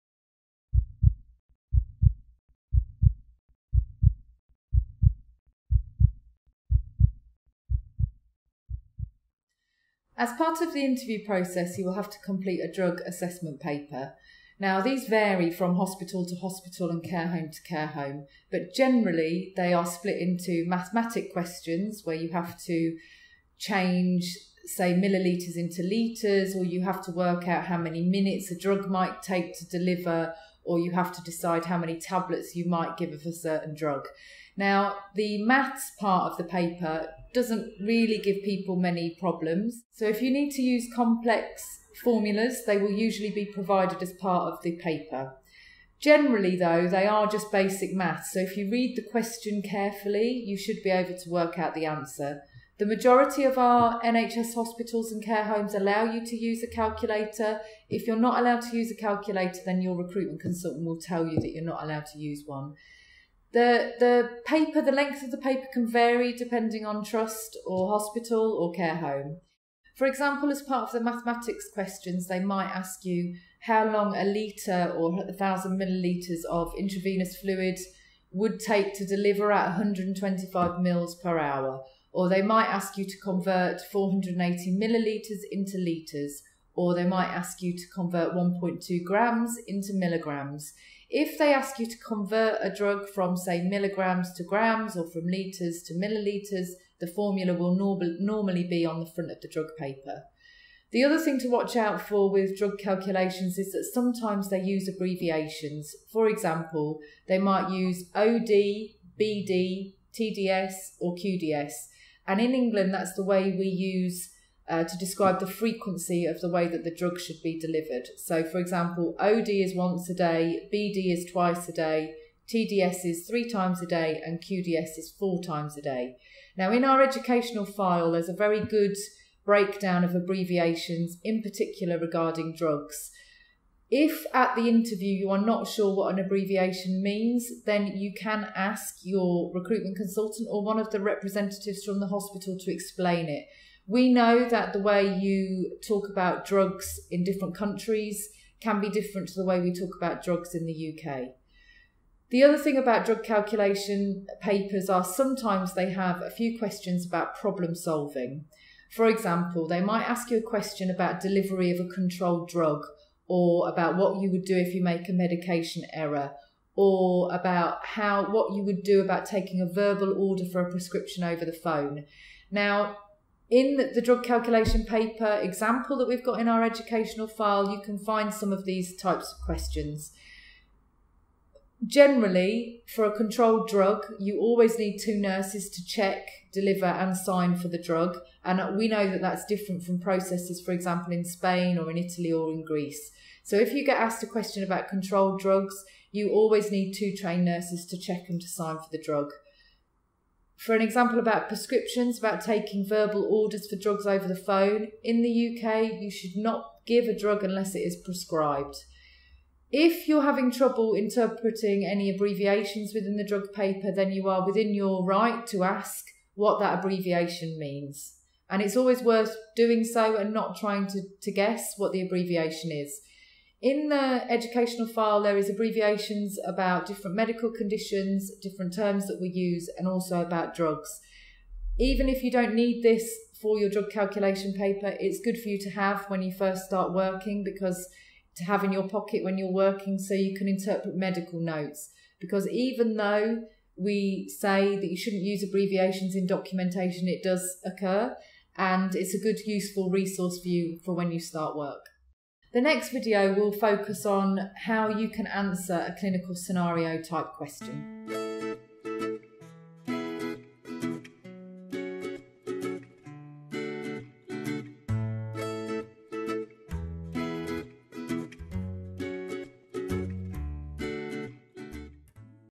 Drug Calculation - Interview preparation for nurses 05
In this video Jennie explains all you need to know about the drug calculation paper that most NHS hospitals and nursing homes use.